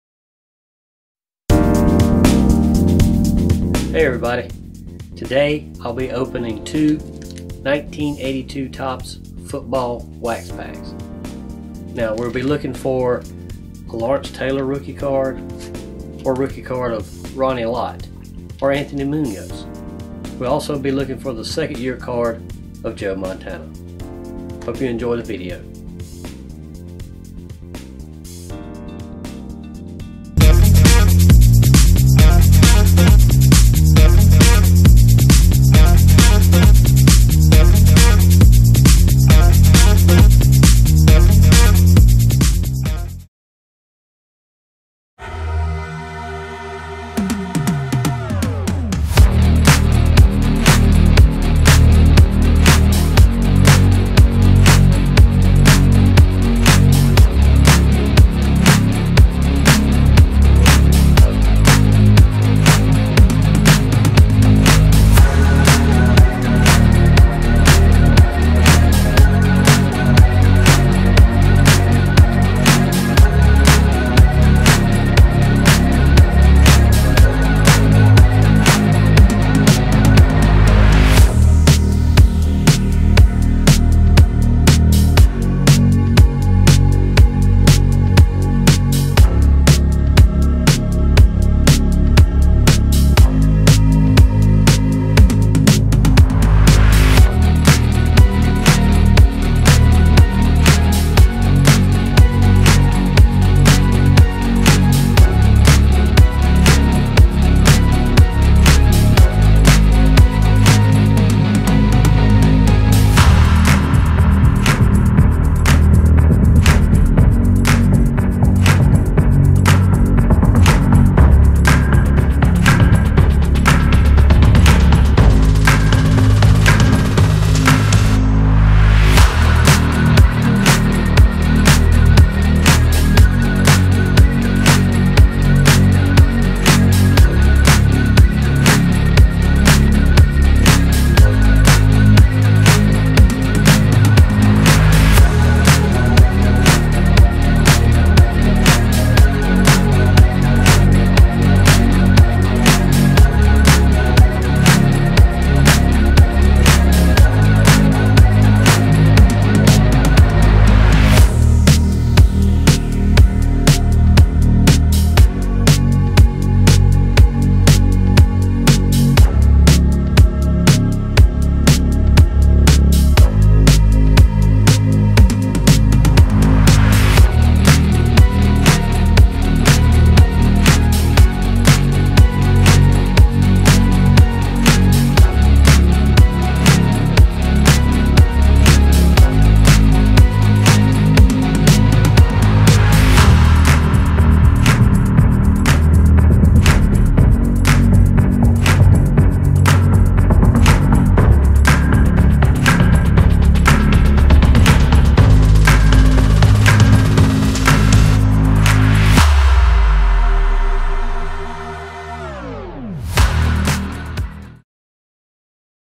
Opening Two 1982 Topps Football Wax Packs
Hoping for some key rookie cards in these two 1982 Topps football wax packs!